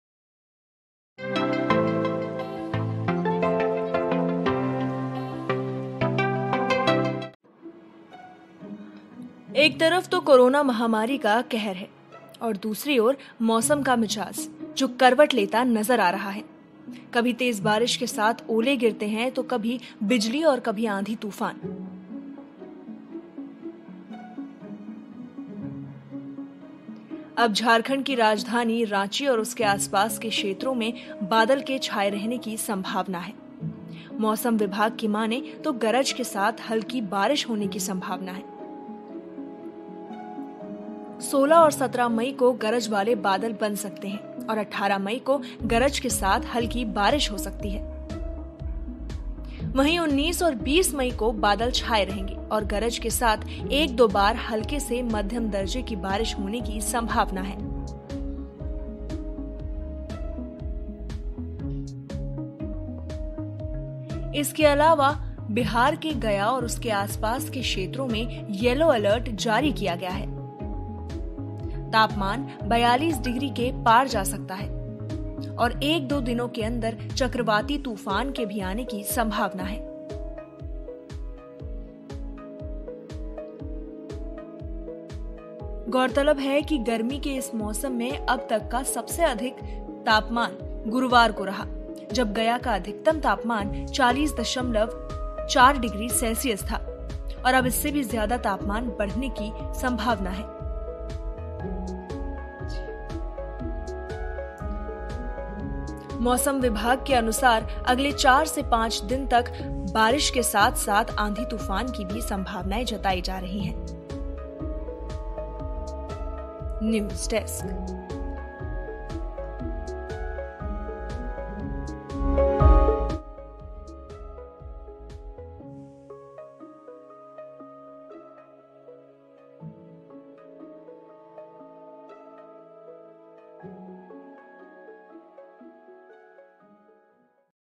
मौसम विभाग ने Gaya में जारी किया Yellow Alert, Jharkhand में भी बारिश की संभावना
झारखंड की राजधानी रांची और उसके आस पास के क्षेत्रों में बादल के छाए रहने की संभावना है. मौसम विभाग की माने तो गरज के साथ हल्की बारिश होने की संभावना है. 16 व 17 मई को गरज वाले बादल बन सकते और 18 मई को गरज के साथ हल्की बारिश हो सकती है.. वहीं 19 व 20 मई को बादल छाए रहेंगे और गरज के साथ एक-दो बार हल्के से मध्यम दर्जे की बारिश होने की संभावना है.